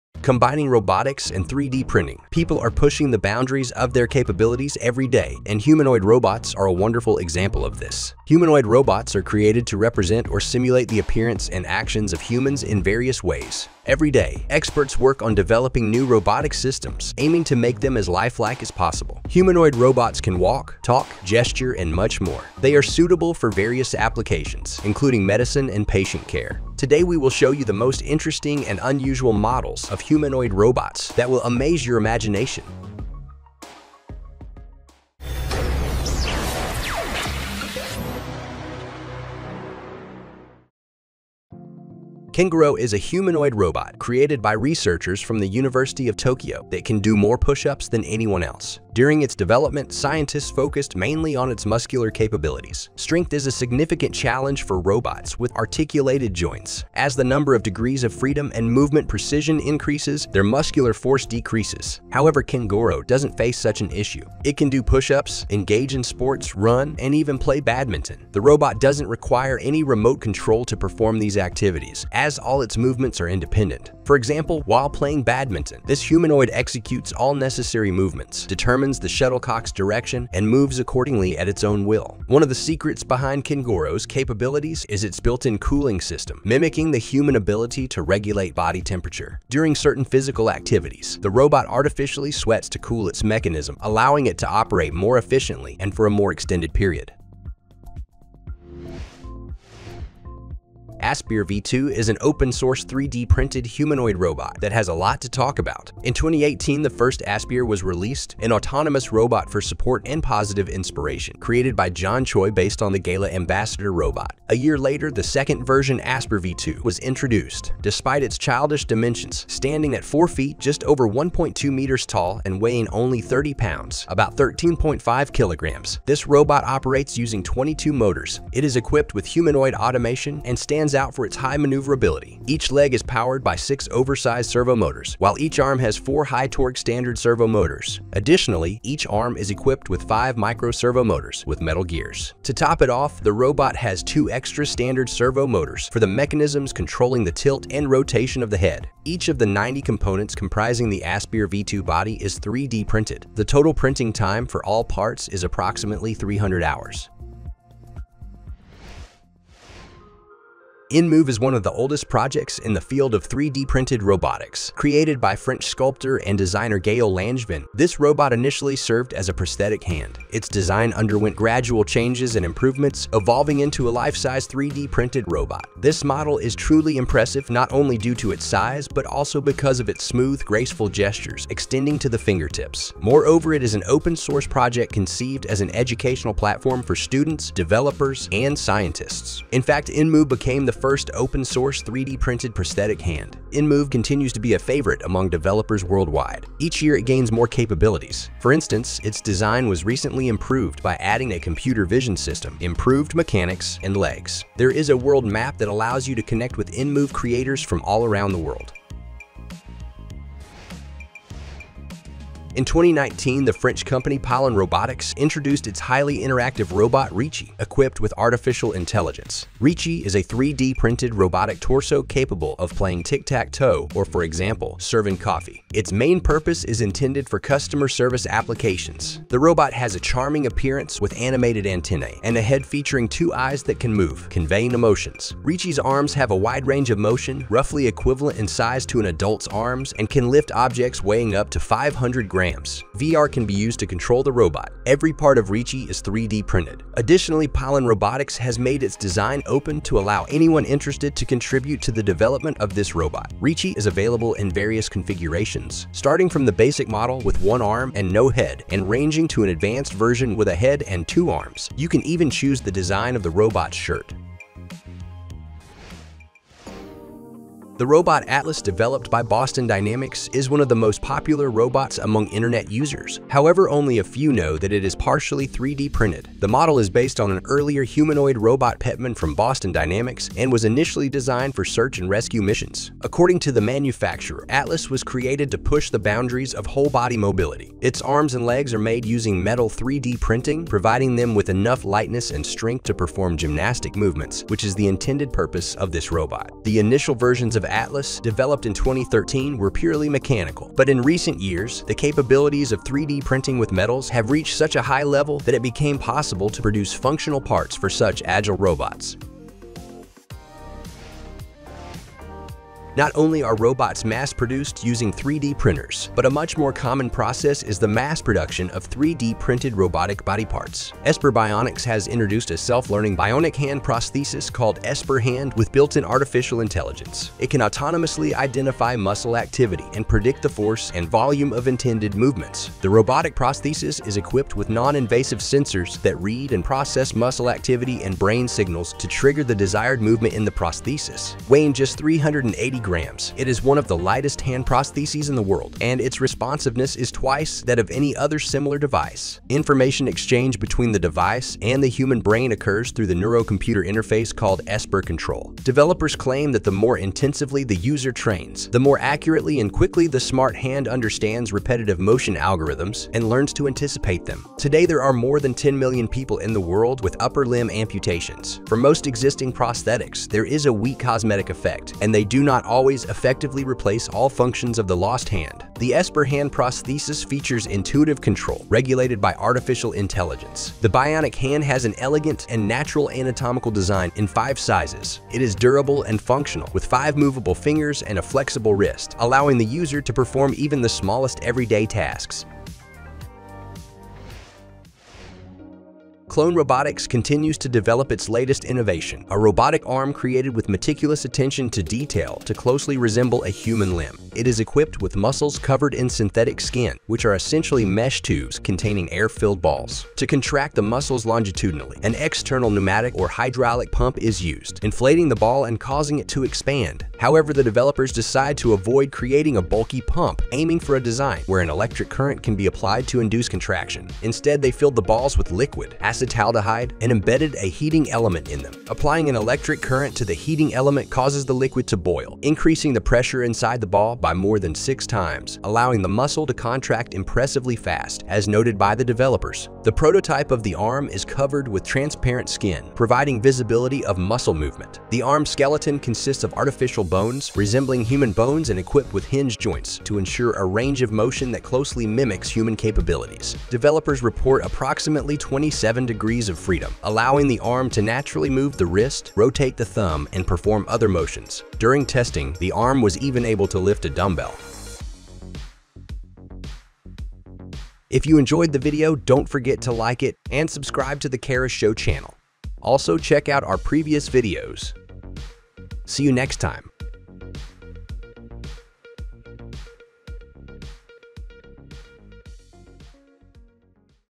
Ultra-Realistic 3D Printed Robots have Begun to Integrate into Human Society
3D printing robots opens up new horizons in the creation of modern automated devices. Thanks to 3D printer technology, robots can be designed with intricate geometric shapes and structures, ensuring optimal performance. This approach also reduces manufacturing costs and allows for the creation of customized solutions tailored to specific tasks, from customer service robots to medical devices and beyond. Thus, 3D printing of robots becomes a key factor in the evolution of modern robotics.